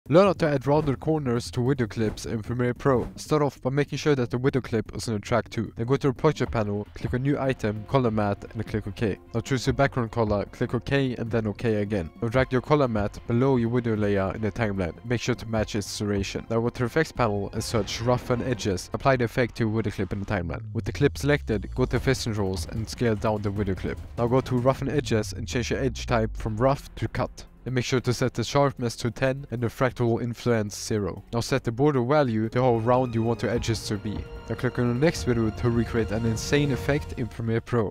How To Make ROUNDED Corners on Video In Premiere Pro
How to add rounded corners to video clips in adobe premiere pro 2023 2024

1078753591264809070

Chapters:
00:00 - Add rounded corners to video tutorial
00:17 - Rounded corners effect
00:25 - Rounded corners settings